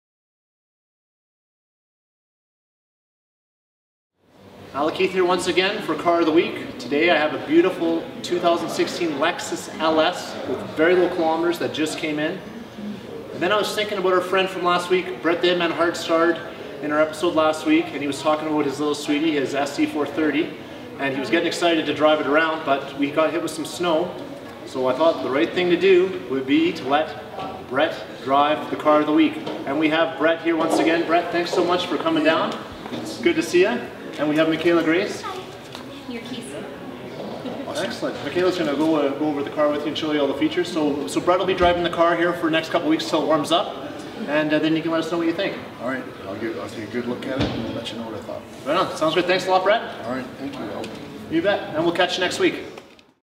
Car of the Week with Lexus of Calgary and Bret Hart!
This week we were excited to welcome back Bret 'The Hitman' Hart, a long time Lexus of Calgary guest in the video.

The vehicle this week is a 2016 Lexus LS 460 F SPORT, stock 170263A.